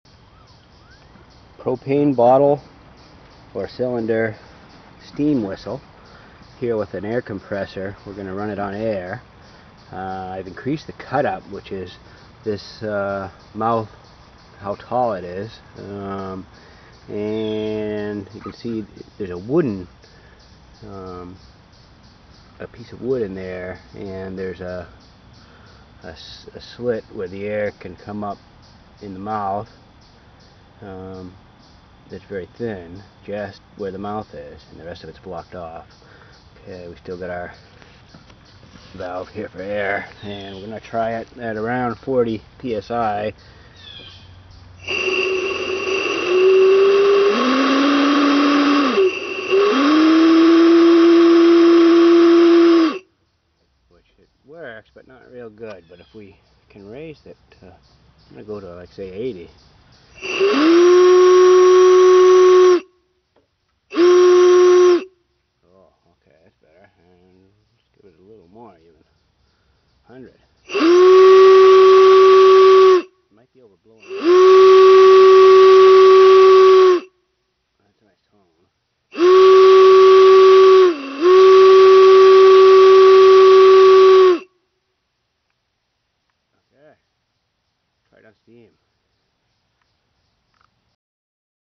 Steam Whistle on Air
DIY Steam or Air Whistle.  Made from a used propane cyliner, actually a couple of them.